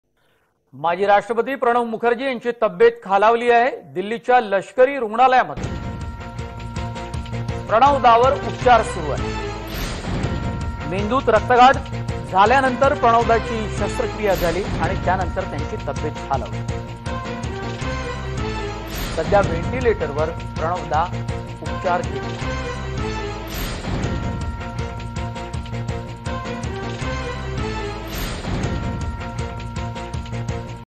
प्रणबदांची तब्येत खालावली | Marathi News
Pranab Mukherjee's health declines, develops lung infection